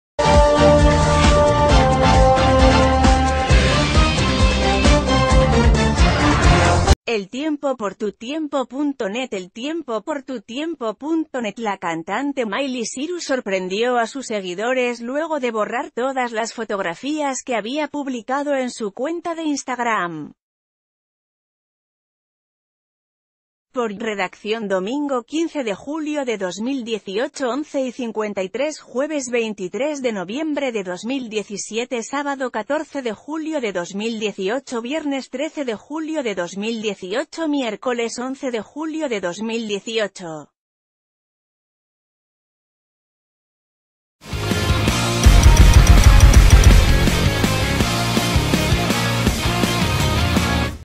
Miley Cyrus borró todas sus fotos de Instagram
El tiempo por Tutiempo.net El tiempo por Tutiempo.net La cantante Miley Cyrus sorprendió a sus seguidores luego de borrar todas las fotografías que había publicado en su cuenta de Instagram. Por: Redacción DOMINGO 15 DE JULIO DE 2018 11:53 Jueves 23 de Noviembre de 2017 Sábado 14 de Julio de 2018 Vi...